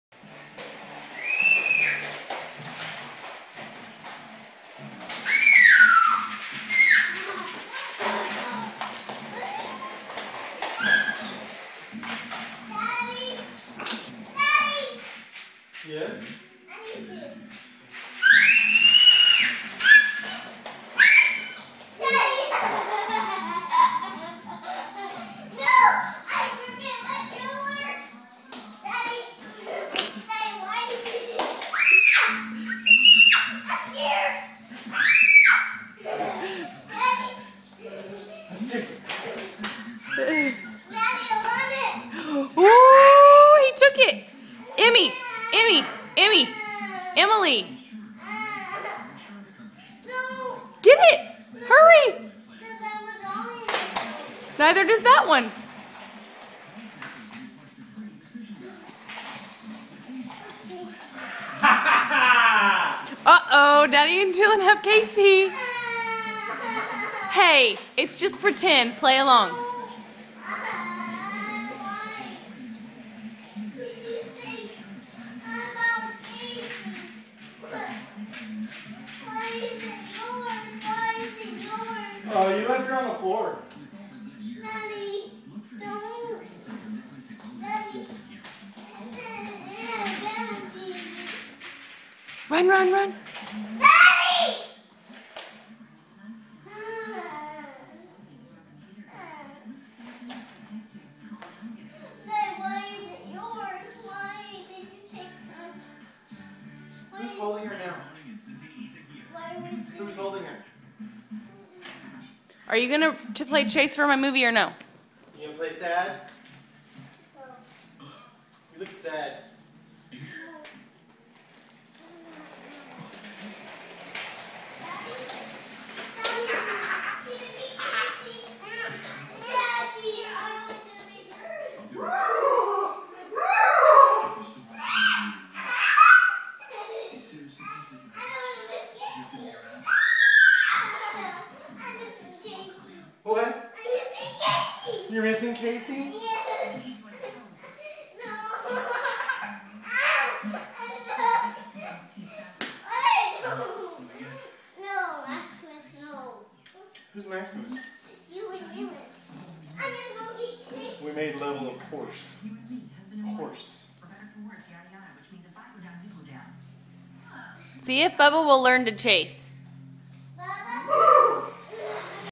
Emmy Plays Chase
She asked me to take a movie of her playing chase with daddy! It's all fun and games until you don't follow her rules!!